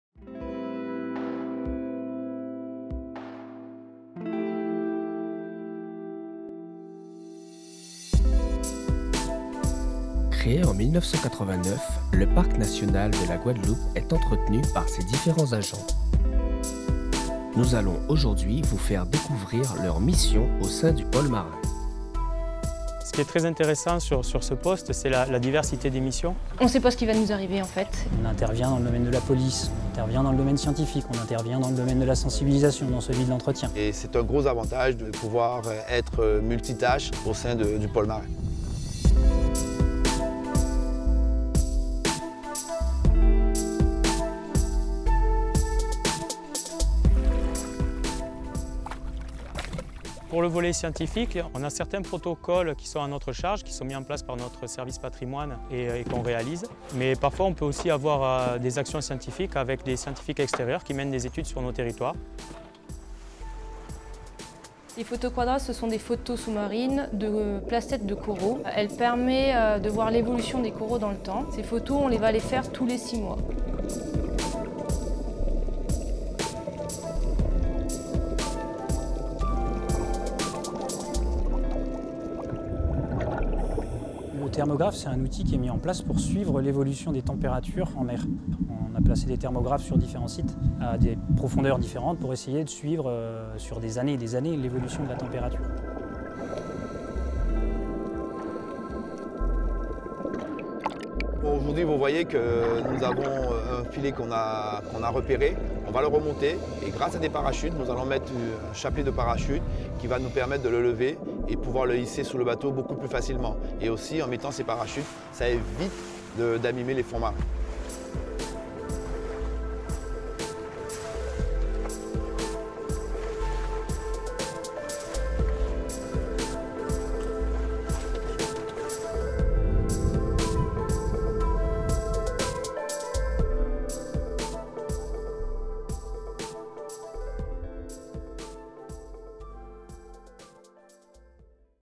Présentation des métiers du pôle Marin
Vidéo réalisée par les BTS audiovisuel du Lycée de Pointe Noire, cette vidéo présente le métier d'agent de terrain en milieu marin dans le Parc national de la Guadeloupe.